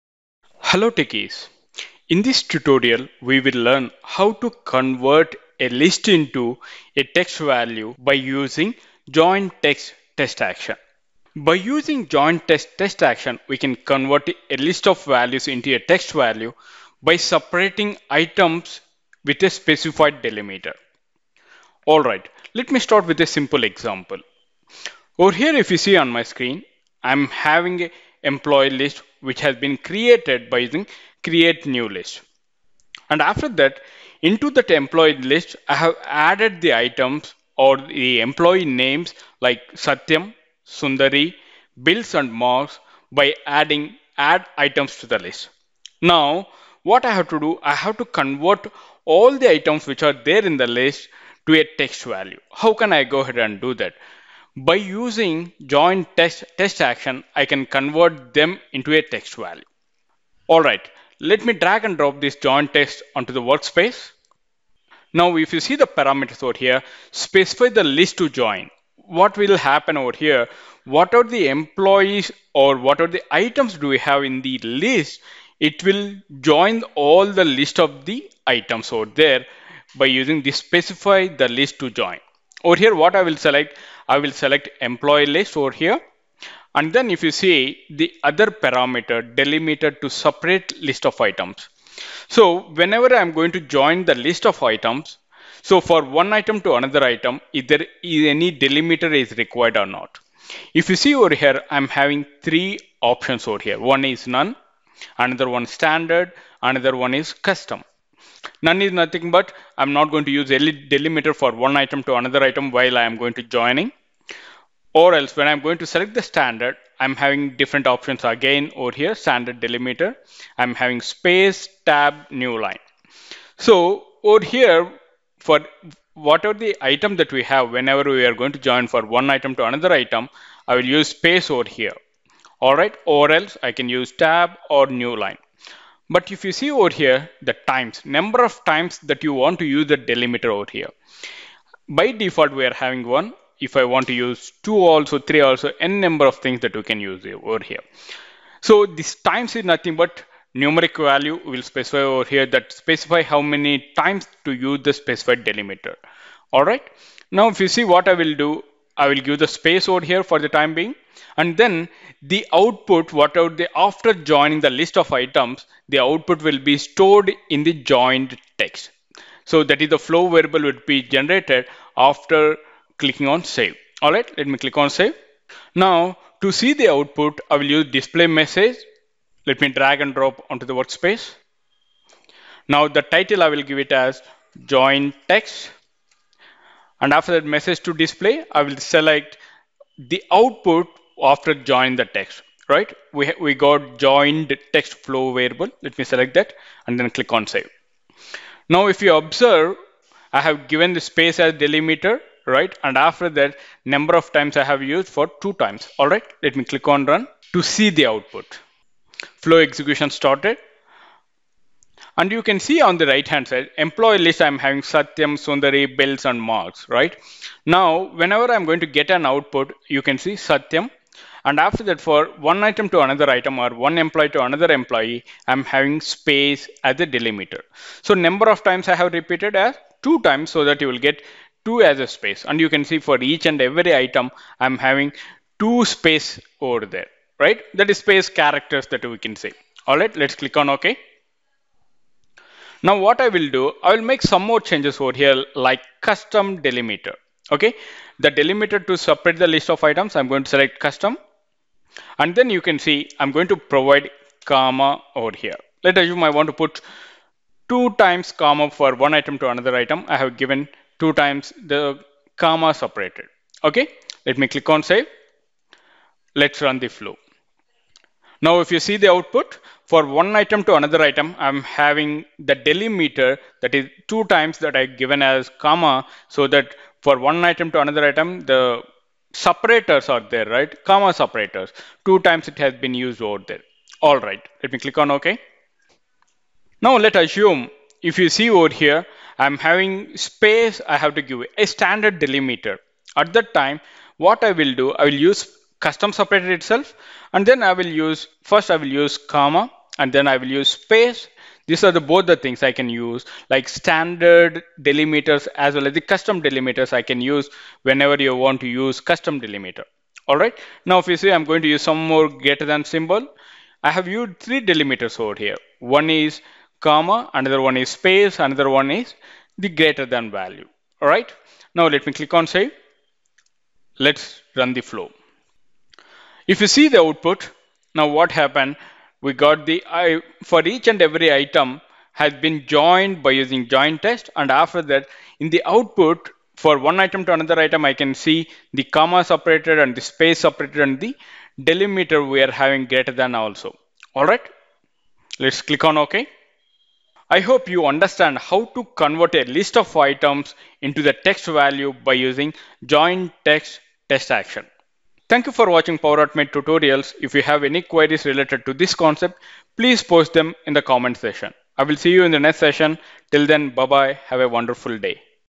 Power Automate Desktop : How to work with "Join Text" Action (Text Actions)
In this Module, we will learn
How to work with "Join Text" Action (Text Actions) in power automate

Join Text : Converts a list into a text value by separating its items with a specified delimiter

Table of Contents:
00:00 Introduction of "Join Text"
01:04 How to work with "Join Text"(Standard Delimiter)
04:02 How to work with "Join Text"(Custom Delimiter)